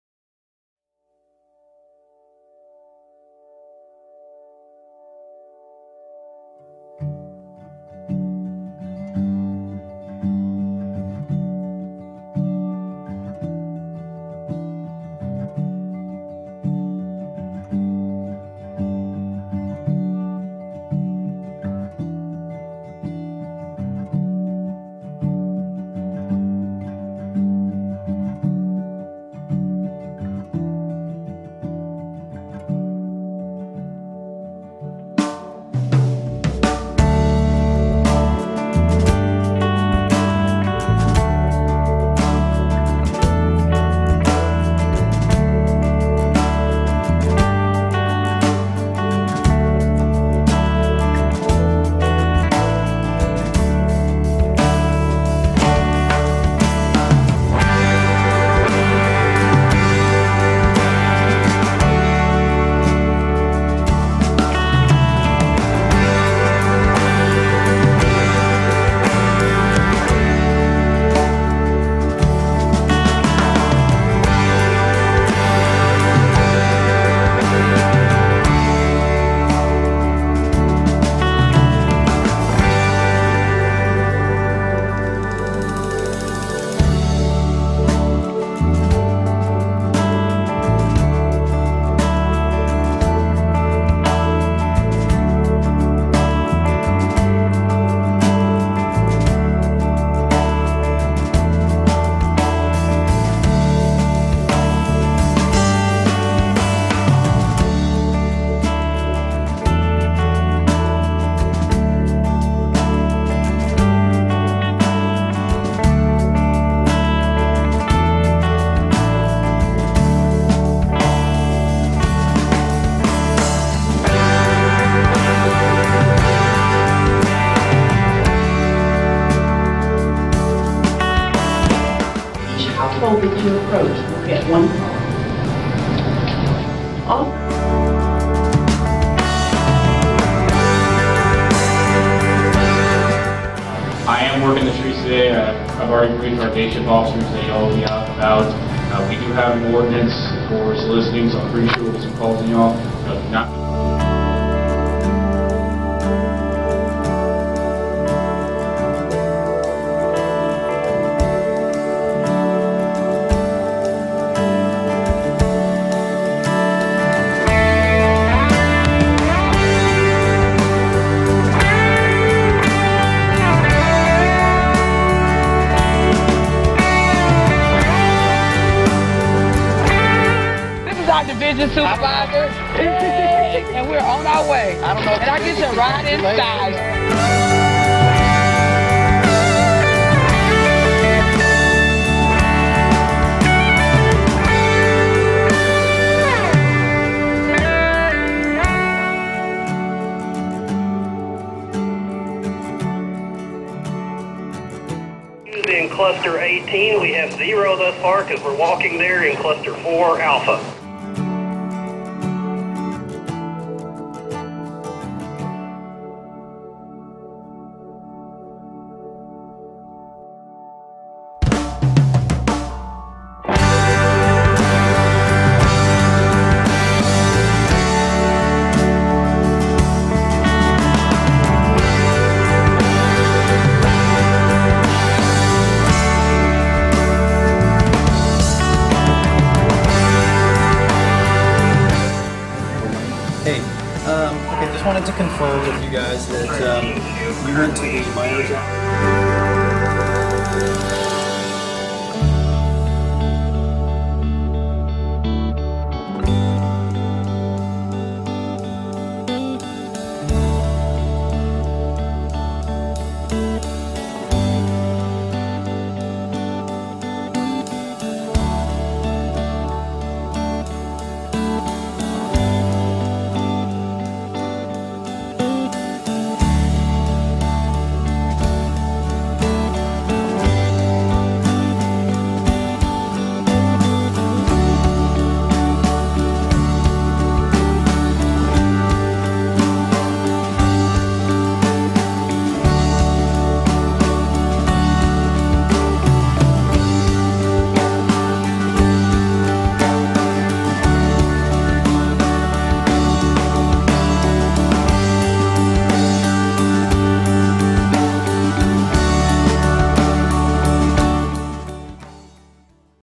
Fort Bend County CASPER- Rosenberg, TX
The Community Assessment for Public Health Emergency Response (CASPER), can be used by public health and emergency management officials to determine the health status and basic needs of the affected community in a quick and low-cost manner. Gathering health and basic needs information.

Fort Bend County HHS used a pre-disaster CASPER to evaluate "How Prepared" jurisdictions are and close any gaps in community preparedness through education and training.